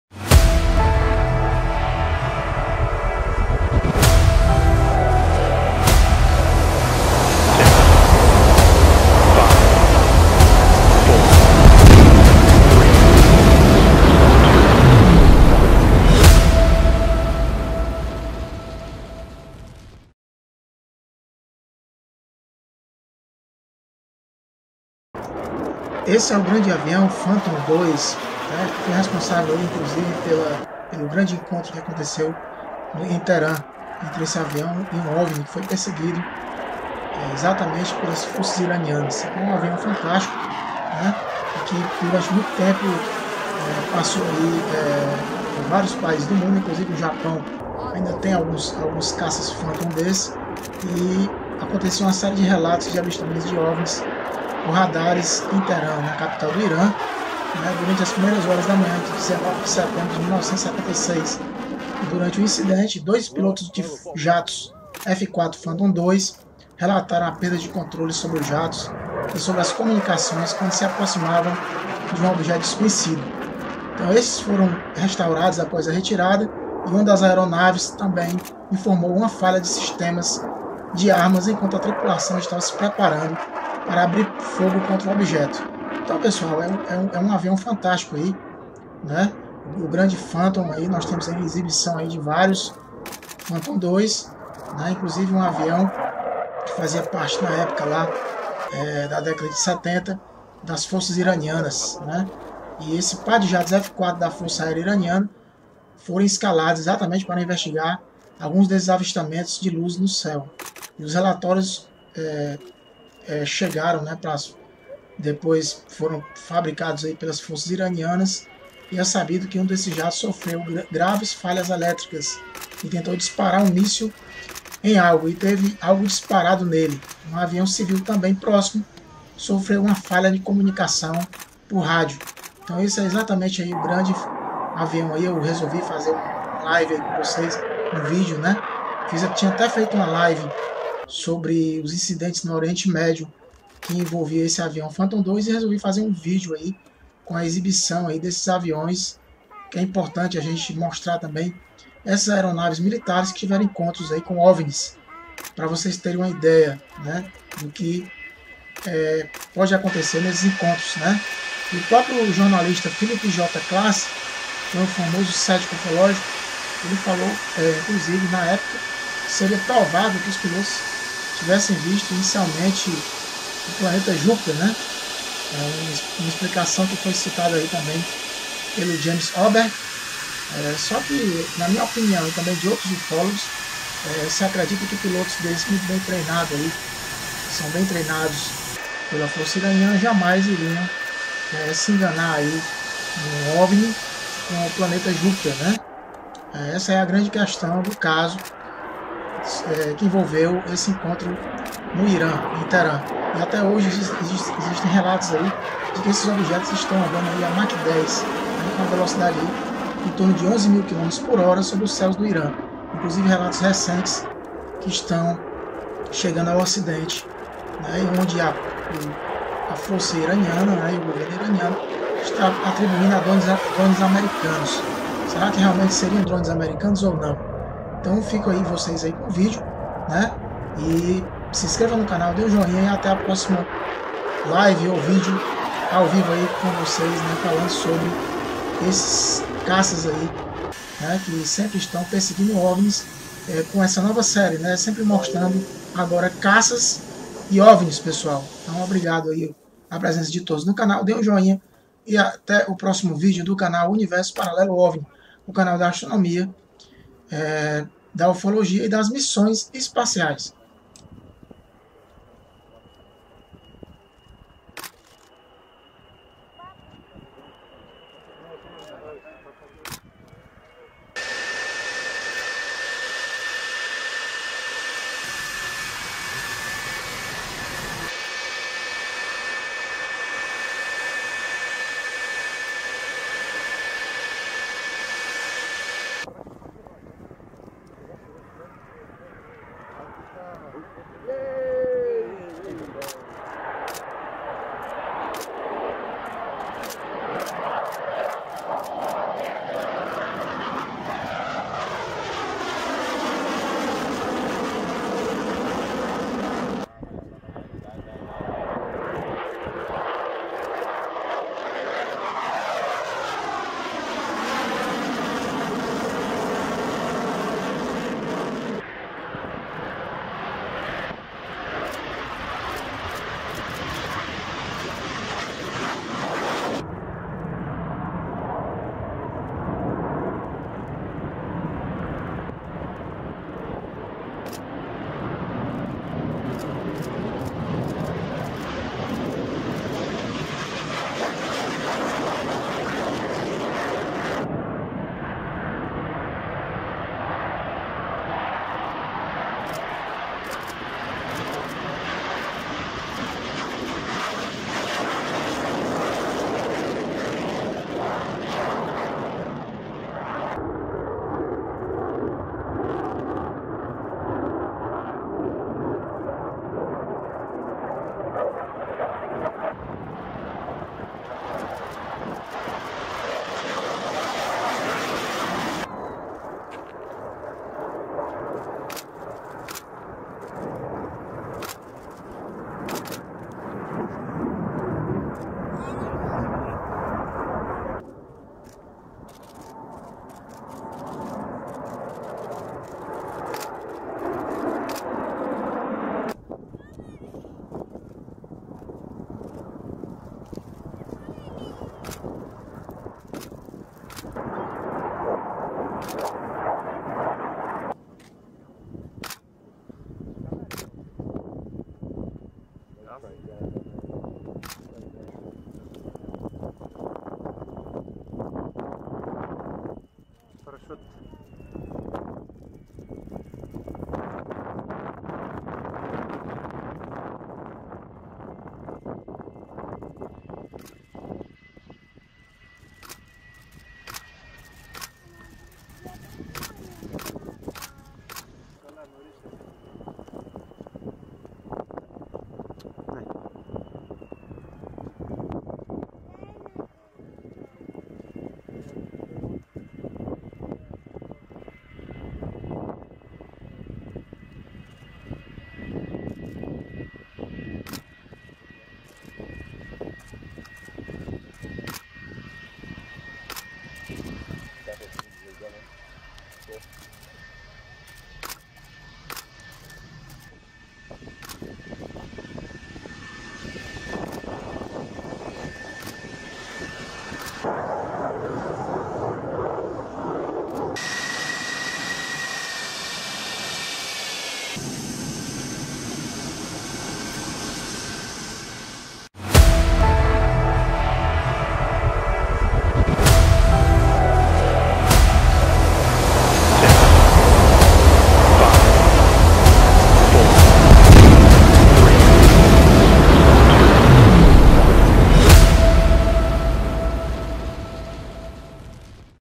O F-4 Phantom II perseguiu OVNIS no Irã.
F-4 Phantom II é um caça-bombardeiro de alto desempenho, capacitado para operar em todas as condições meteorológicas e de luminosidade.O Incidente de OVNI em Teerã foi uma série de relatos de avistamentos de OVNIs por radares e visualizados em Teerã, a capital do Irã, durante as primeiras horas da manhã de 19 de setembro de 1976. Durante o incidente, dois pilotos de jatos F-4 Phantom II relataram a perda de controle sobre os jatos e sobre as comunicações quando se aproximaram de um objeto desconhecido.